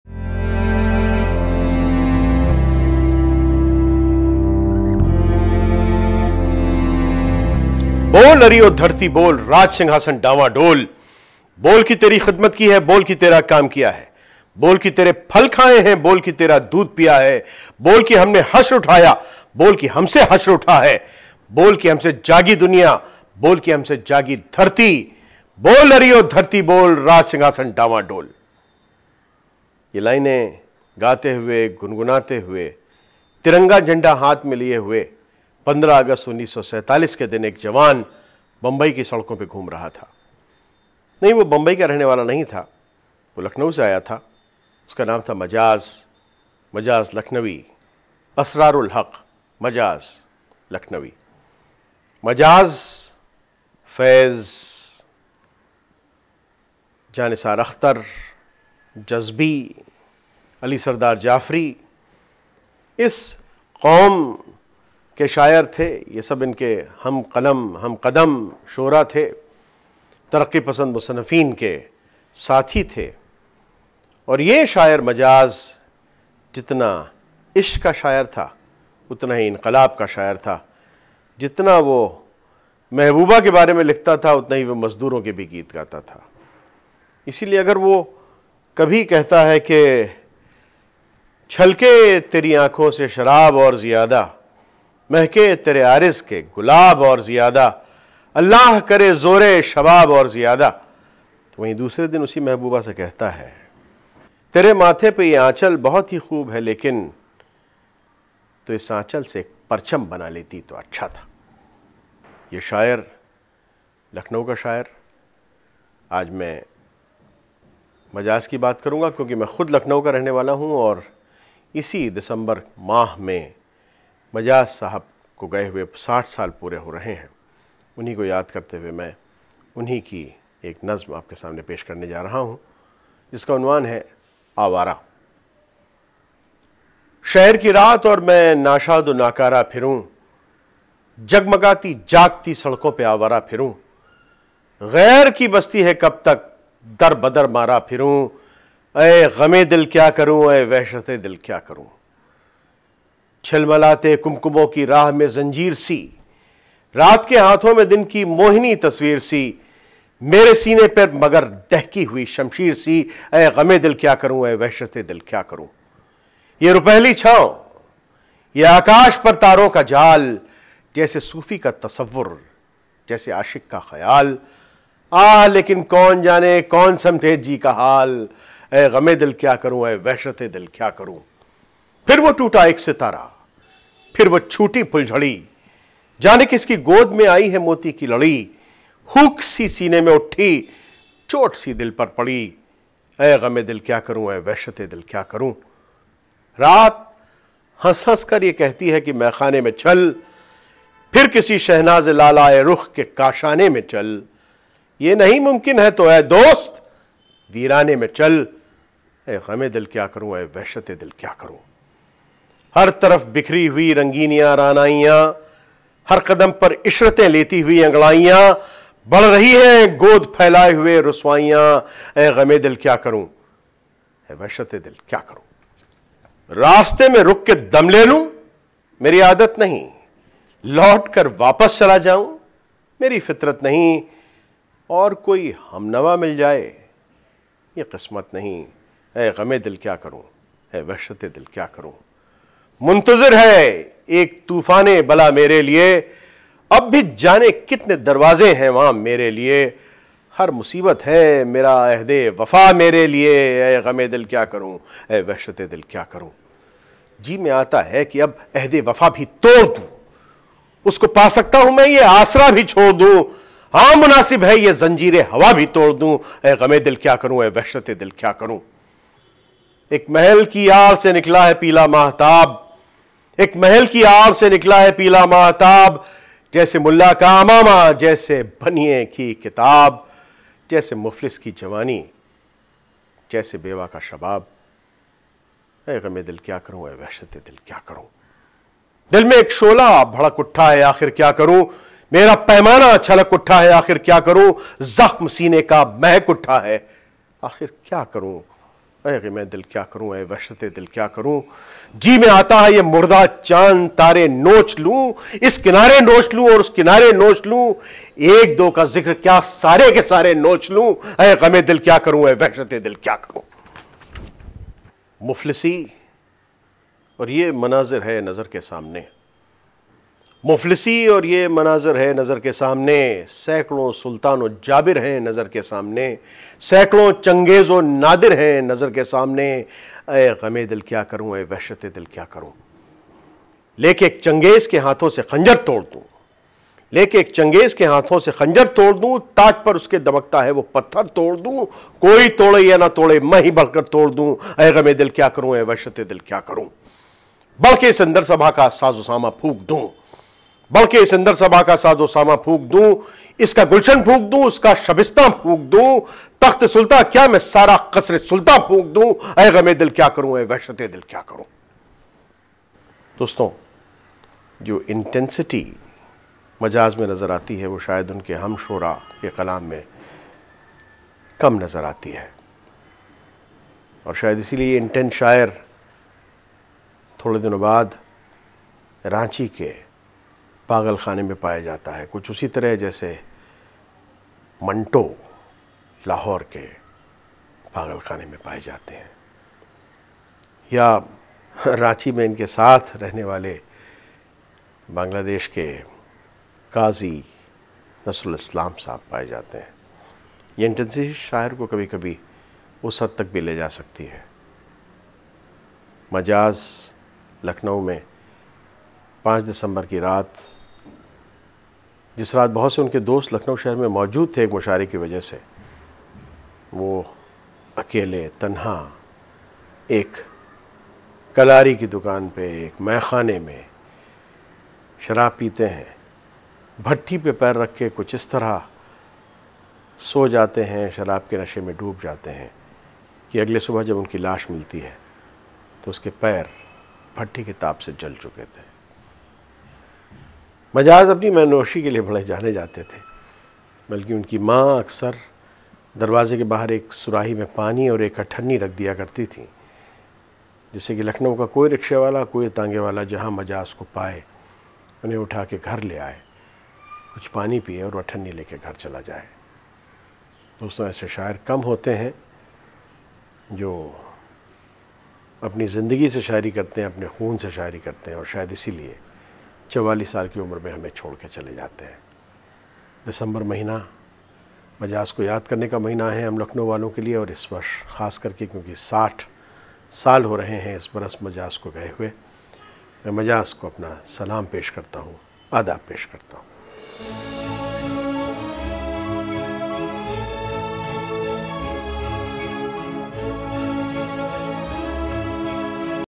Urdu Studio : Awara : आवारा - शहर की रात : Atul Tiwari with Manish Gupta in Hindi
Asrar ul Haq Majaz (Urdu: مجاز لکھنوی, Hindi: मजाज़ लखनवी) (19 October 1911 – 5 December 1955) was an Indian Urdu poet. He was known for his romantic and revolutionary poetry. He composed ghazals and nazms in Urdu.

Born as Asrar ul Haq in Rudauli, Bara Banki, UP, India. He received his early education in Lucknow and Agra, and did his B.A. at Aligarh Muslim University. He considered Fani Badayuni as his 'ustad'.

He hails from a family of poets and litterateurs. Muztar Khairabadi, one of the great masters of Urdu poetry and Usman Harooni, an early modern Sufi poet, were among his ancestors. He had a sister Safiya, who married Jan Nisar Akhtar, father of Javed Akhtar.